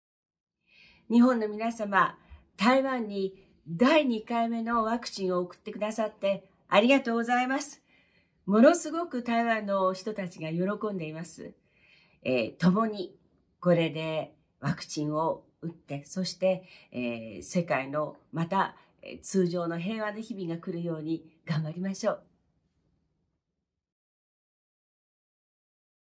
ジュディ・オングより、台湾へのワクチン提供の御礼です。
日本から台湾に向け、2021年7月8日に2回目のワクチン提供をしていただきました。
台湾出身であるジュディ・オングより、日本のみなさまに御礼申し上げます。
Judy Ongg's appreciation message for Japan about 2nd-time shipment of the COVID-19 vaccines to Taiwan.